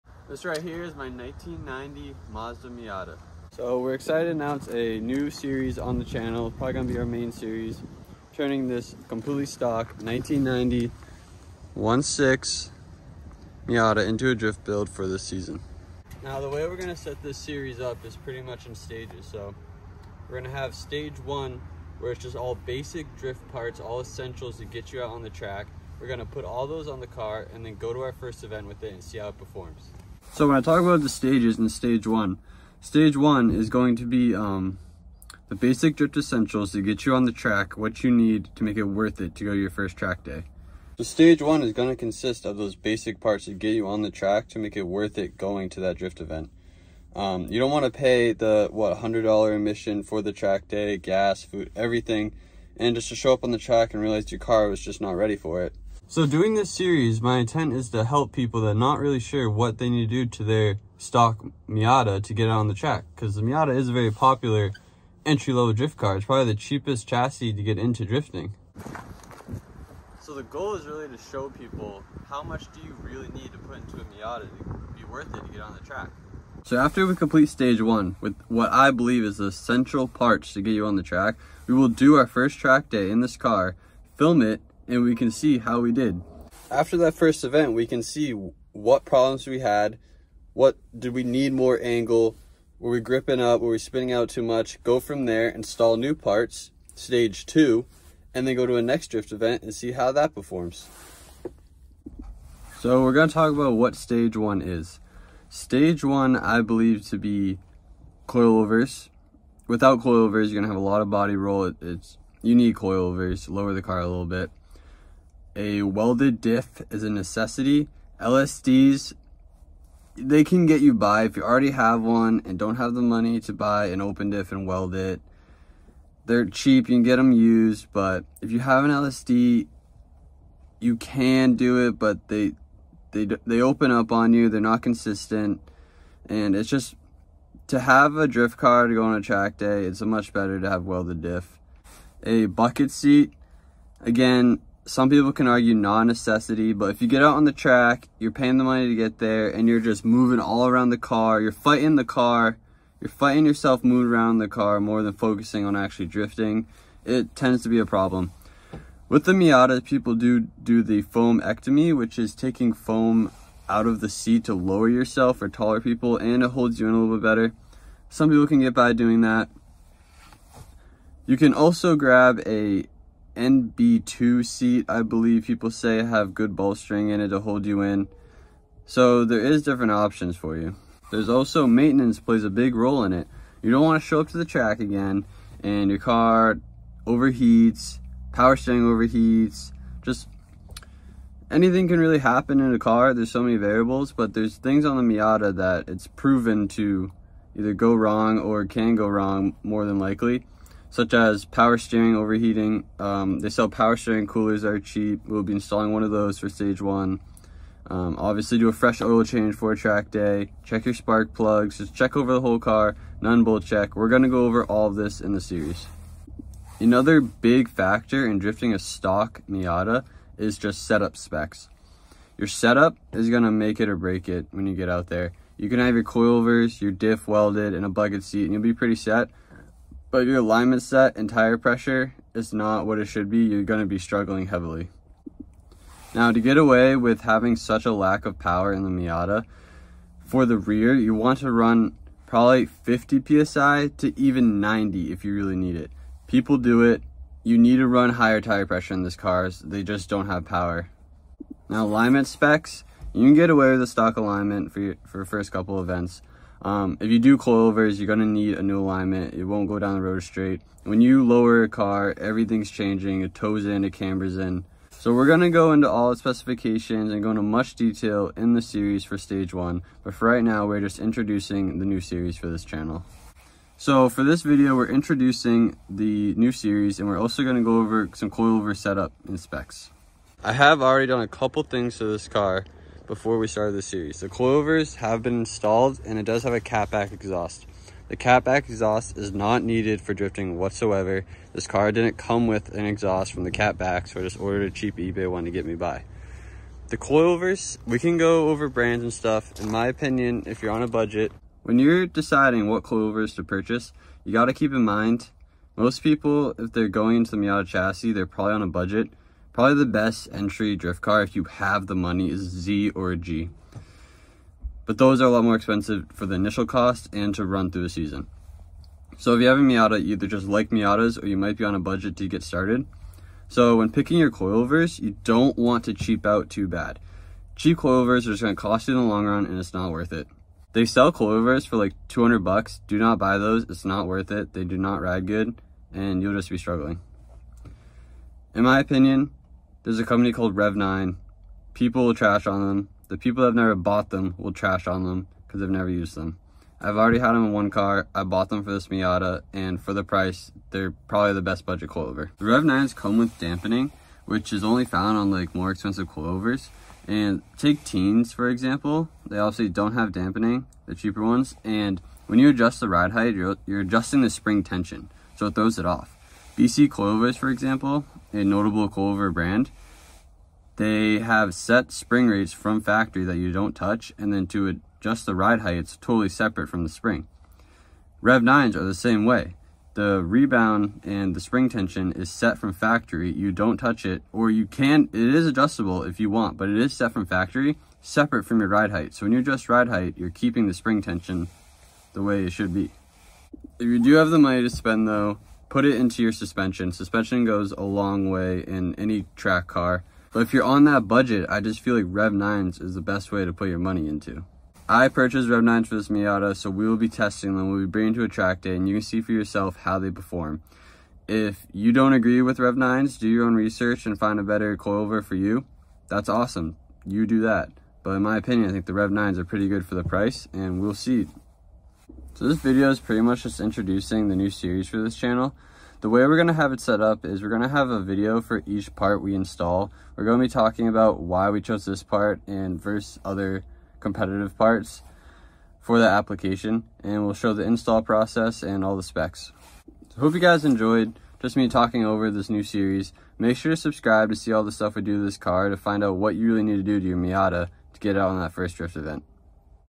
BEST COILOVER FOR MIATA!
Basic video to introduce the new series. We will be going into much detail of each part we install for stage one and showcase how to install and why we chose that specific part.

Follow our Instagram to keep up to date with all are projects and events!